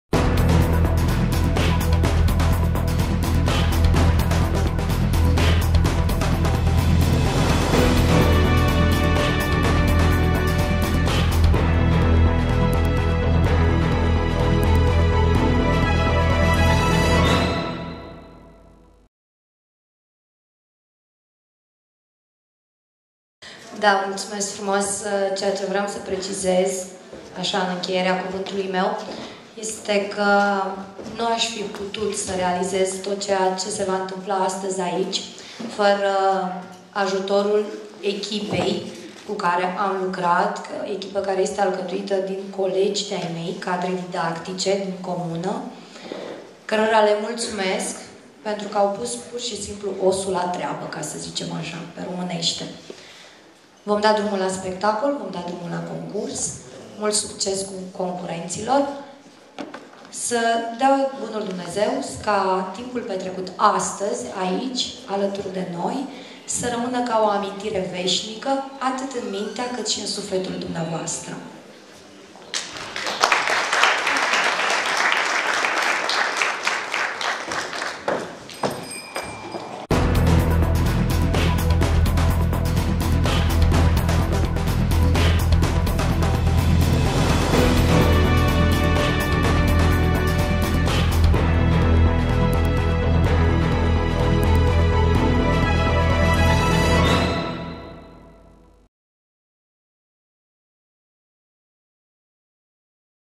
Ana Alina Muncila aduce multumiri echipei pentru realizarea festivalului PODUREANCA
Ana Alina Muncila aduce multumiri echipei pentru realizarea festivalului PODUREANCA, Comuna Poduri, judetul Bacau

Cu stimă,
echipa Exclusiv

Vizionare plăcută !